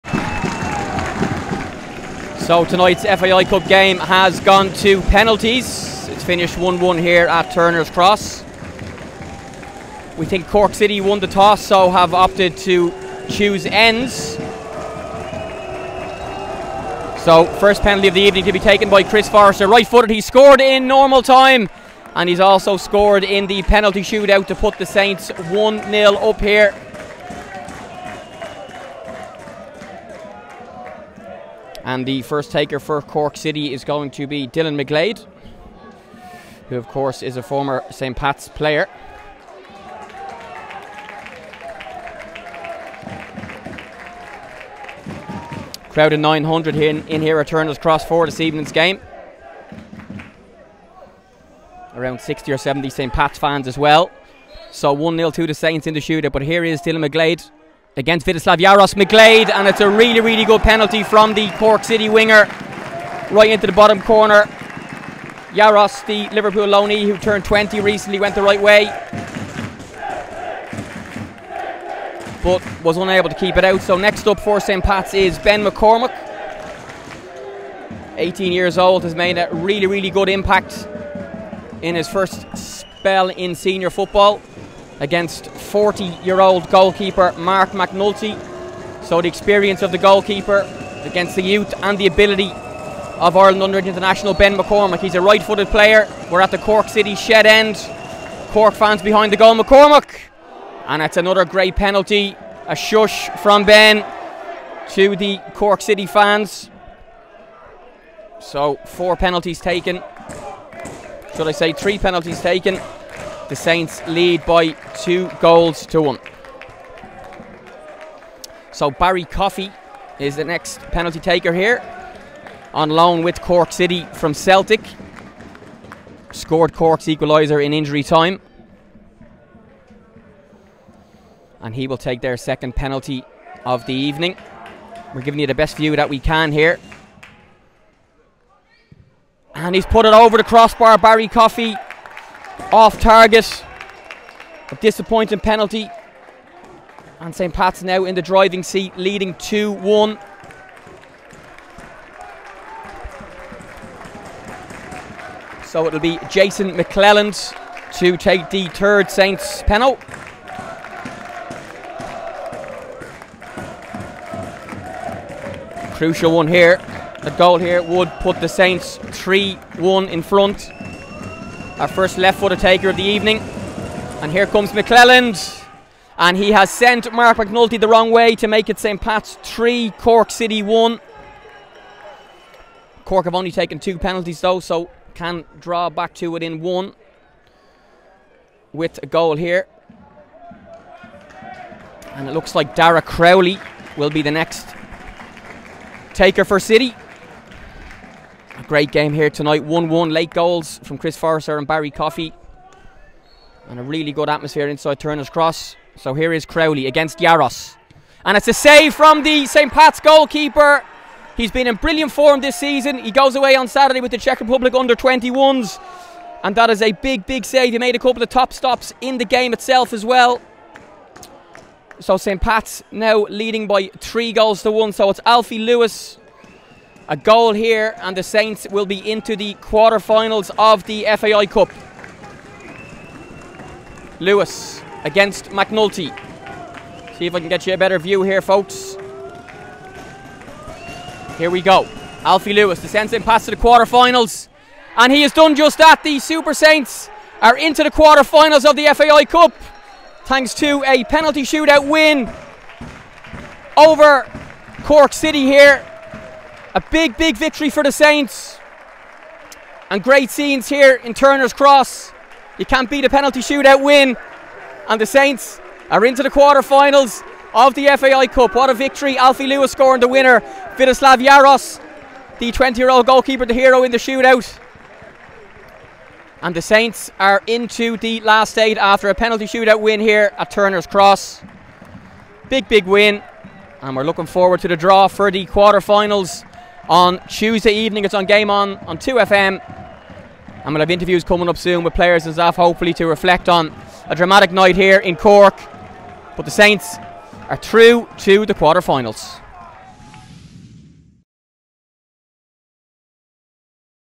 PENALTY SHOOTOUT | Cork City 1-4 Saints | Extra.ie FAI Cup 2nd Round
What a dramatic night at Turners Cross as we advanced to the 1/4 finals of the Extra.ie FAI Cup 🏆

Forrester, McCormack, McClelland & Lewis all scored while Jaros made a crucial stop 😍

Here is the penalty shootout in full with our Jamie Moore on commentary 🎙